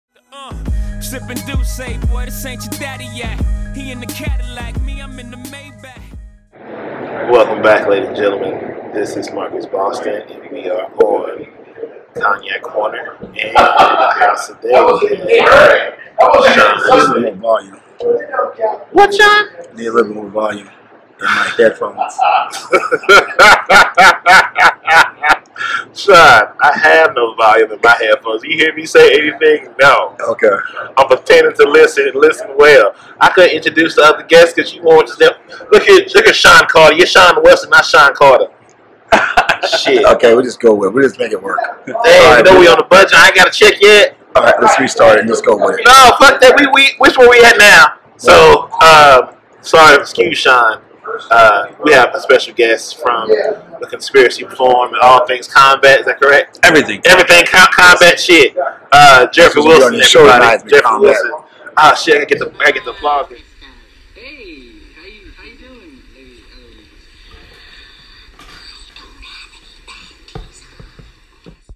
Sean Wesley & Jeffery Wilson : 📉Failure is an Opportunity 📈
in this episode...We have Jeff Wilson of The Conspiracy Farm, ItsMeSpkn2U & Everything Combat Podcast plus Sean Wesley is back in the Gran Cru Cigar Bar @ 1013 Ann Ave 63104.  We talk about all things that moves the podcast culture forward and staying positive with the consequences of our choices.
Jeff shared about how he got into podcasting and how he fell in love with the process. Sean keeps it on brand with overflowing with gratitude and positivity.
Enjoy...Drink up
Podcast location:
Gran Cru Cigars Soulard
1013 Ann ave 63104

Jeffery Podcasts:
The Conspiracy Farm
Everything Combat
ItsMeSpkn2U

Go follow Cognac Corner on IG & Twitter